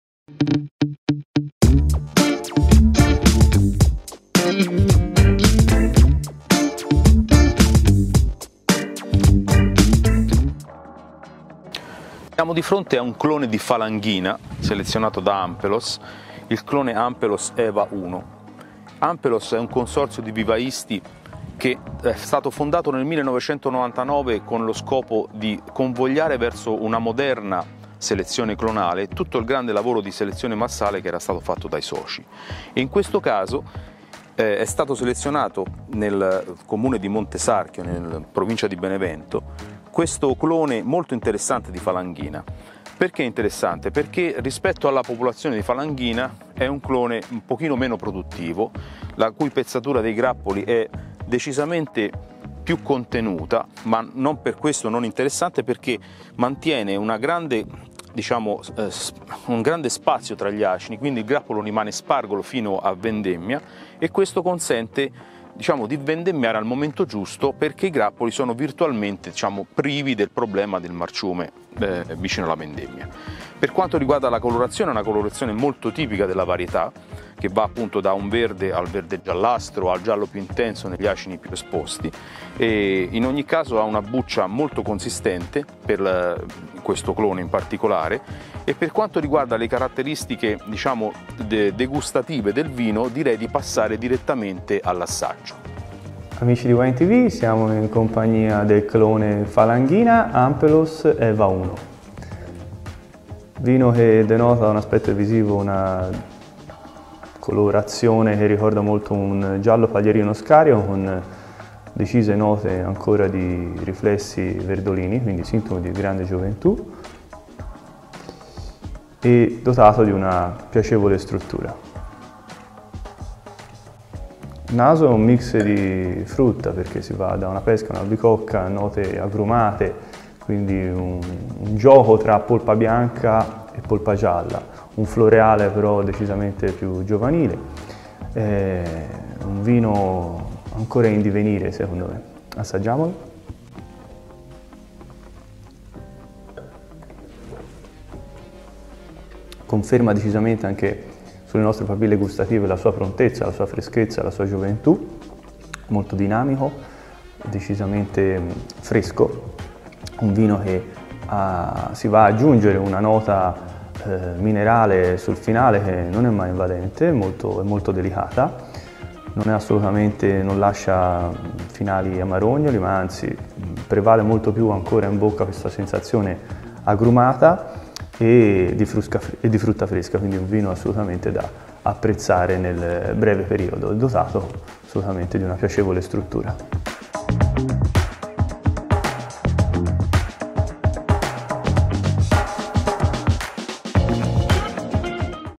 Il mondo dei cloni - Ampelos Falanghina Eva 1
Marco Pierucci e Andrea Galanti ci raccontano il clone di falanghina Ampelos Eva 1